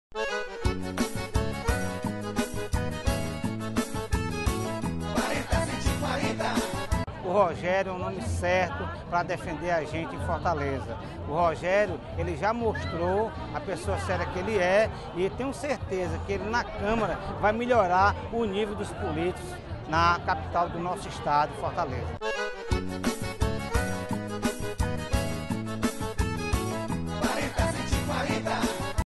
Dr. Israel é Rogério Pinheiro Vereador 40140
Conceituado médico com atuação em Fortaleza, Dr. Israel destaca a seriedade de Rogério Pinheiro, já comprovada em 25 anos de luta por justiça social. Ele vota e pede voto para vereador 40140!!!